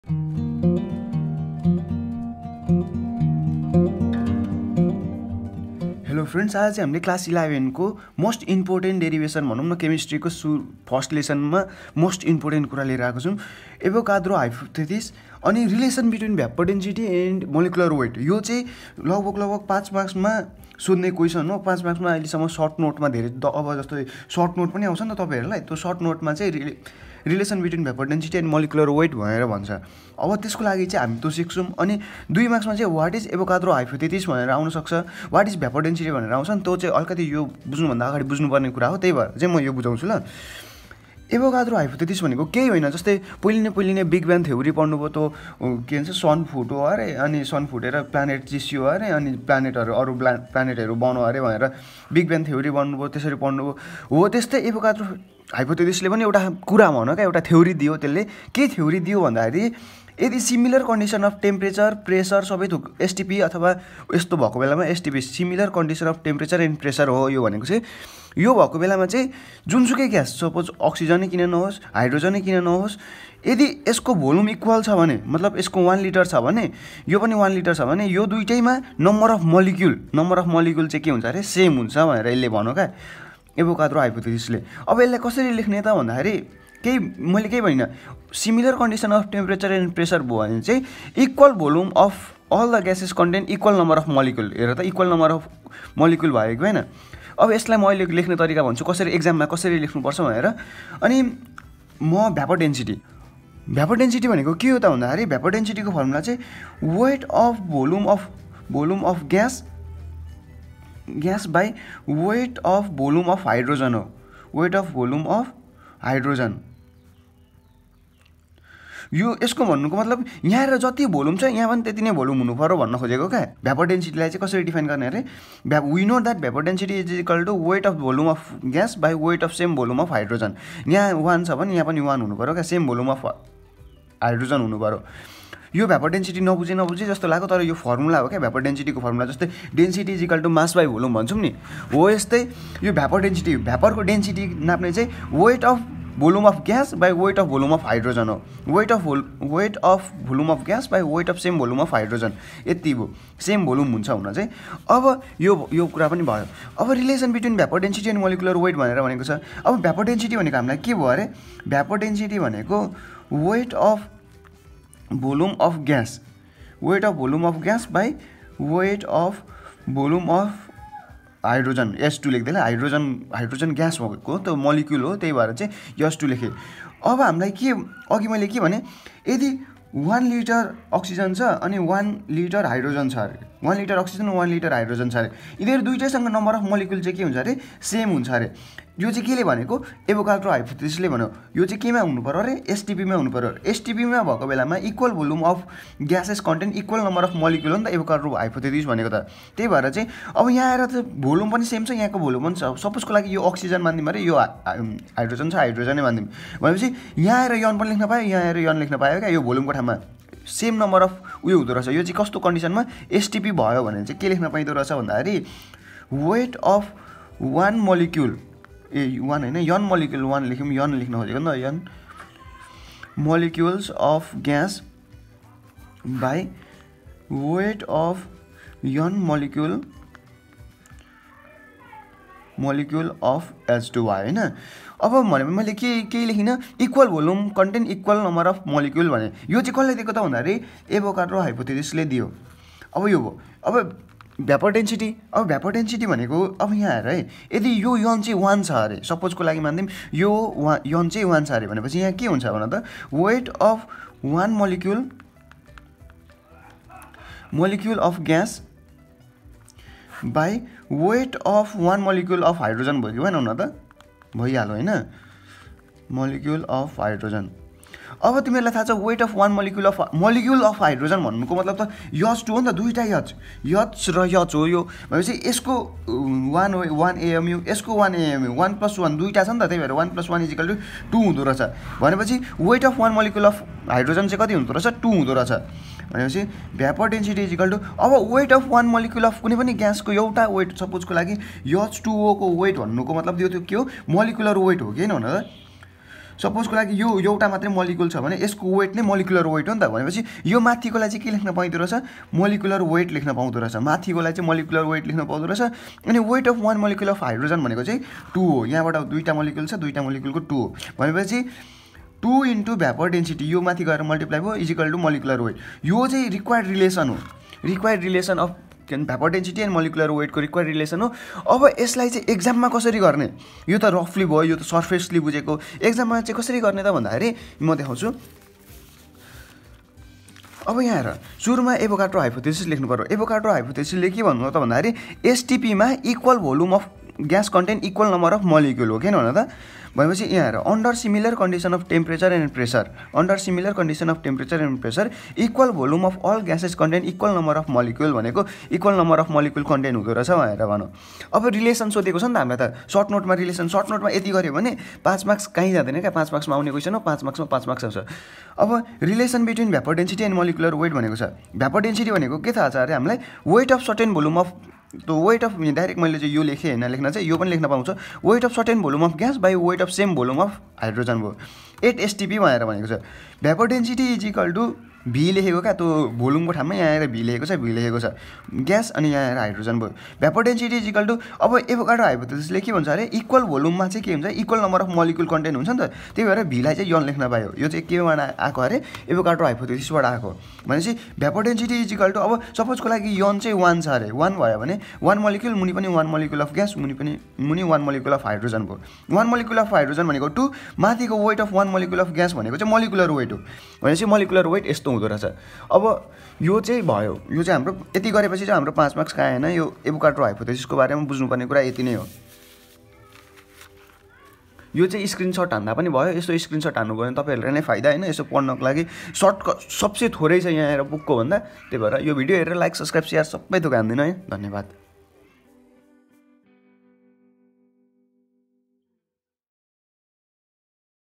Class 11 Chemistry || Avogadro’s Hypothesis || Relation Between Vapour Density & Molecular weight
Hey viewers!
In this video we’re going to learn about Avogadro hypothesis and Relation between vapour density and molecular weight which usually comes in short notes in exam!